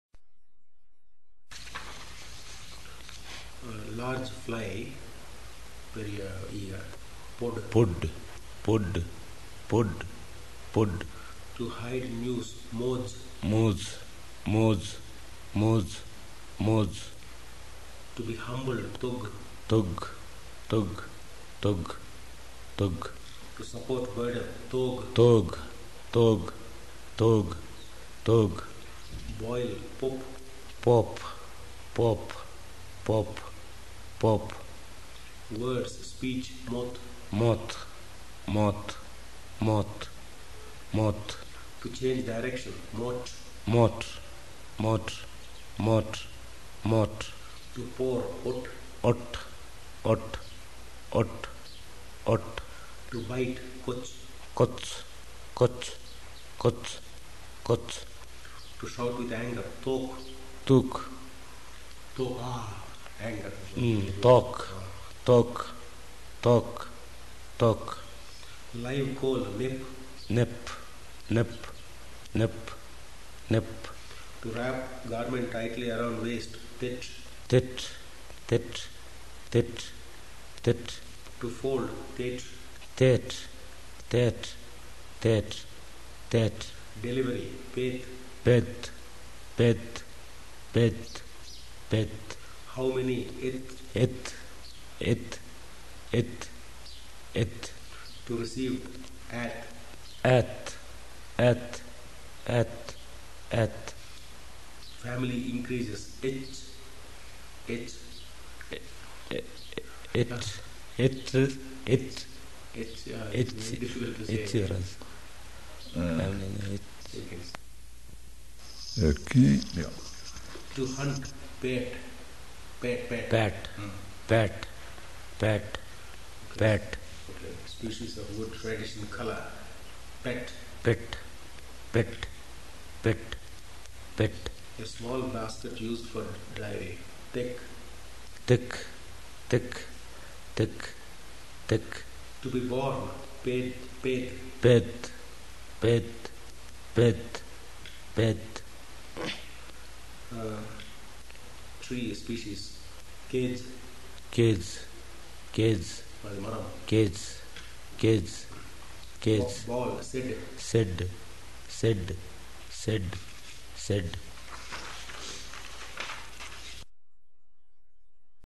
Toda audio: tcx_word-list_1992_08
Toda (tcx) audio: tcx_word-list_1992_08
Language family: Dravidian; Southern Dravidian; Tamil-Kannada; Tamil-Kodagu; Toda-Kota;
Nilgiri Hills, Southern India
Date: 2/7/1992

Description: Language Toda.  tcx_word-list_1992_01.html, enties: 82 - 103

Contributor: Tes
UCLA Phonetics Archive